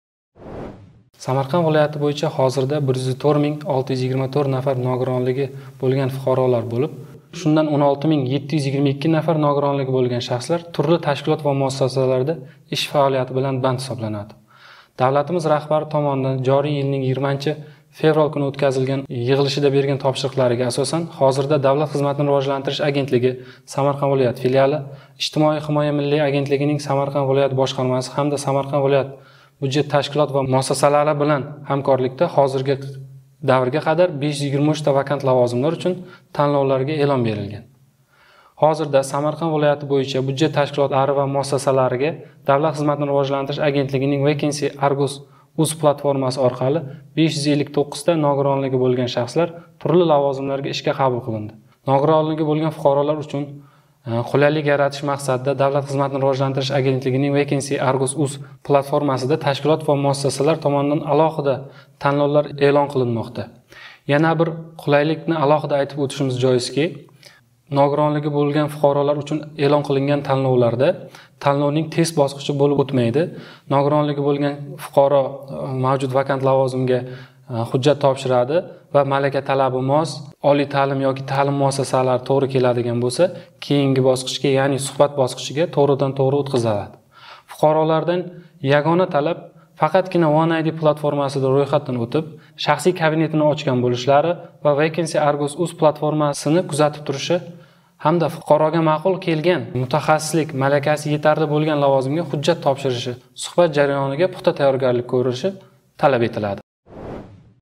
Ногиронлиги бўлган шахслар ҳам ишга тест топшириб кирадими?
Вилояти бўйича 104600 нафар ногиронлиги бўлган фуқаролар бўлиб, шундан 16722 нафари турли ташкилот ва муассасаларда иш билан банд. Шу кеча-кундузда яна 523 та вакант лавозимлар учун танловлар эълон қилинган.

Хўш, бу иш ўринларига ногиронлиги булган шахсларни қабул қилиш тартиби қандай? Улар ҳам барча каби тест имтиҳонидан ўтадими? Шу ва бошқа саволларга Ижтимоий ҳимоя миллий агентлиги вилоят бошқармаси шуъба мудири Камолиддин АБДУЛЛАЕВ жавоб берди.